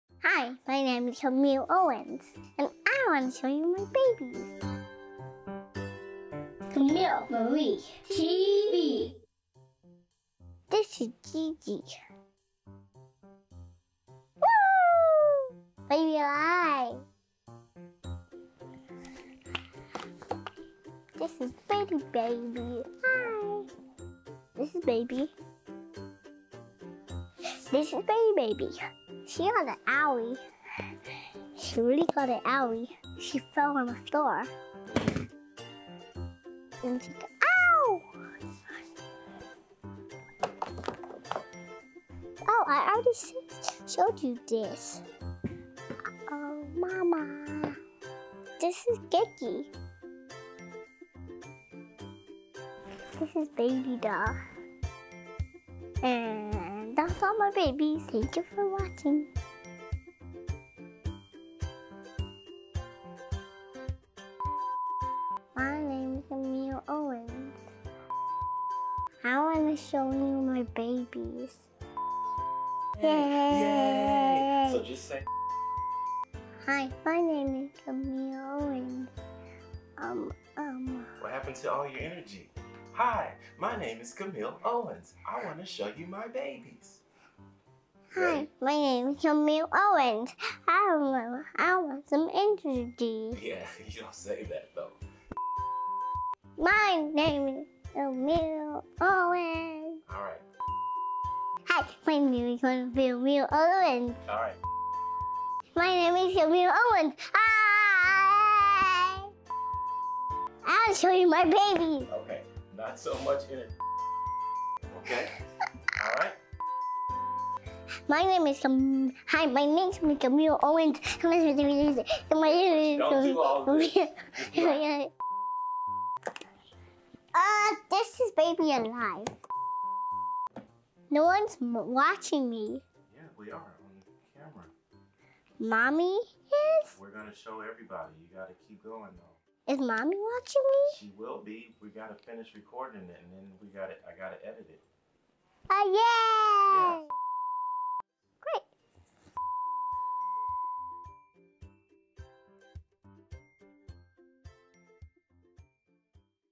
Camille Marie TV (Episode 1) "My Babies" - Toddler introduces you to some of her baby dolls
Camille Marie Owens introduces you to a few of her baby dolls in the debut episode of Camille Marie TV.